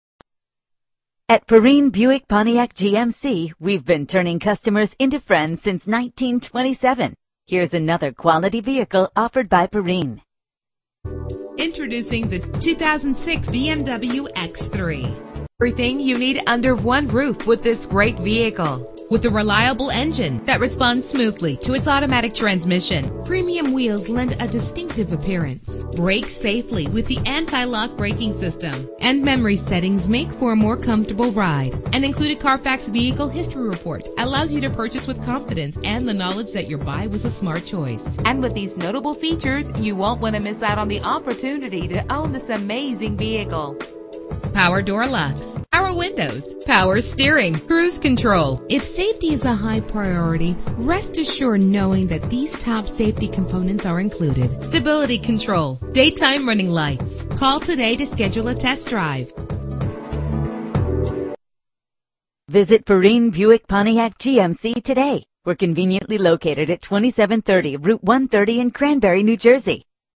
2006 BMW X3 - Cranbury NJ
Year: 2006
Make: BMW
Model: X3
Trim: 3.0i Sport Utility 4D
Engine: 6 cylinder 3.0 liter
Transmission:
Color: Black
Mileage: 75834
Address:
2730 Route 130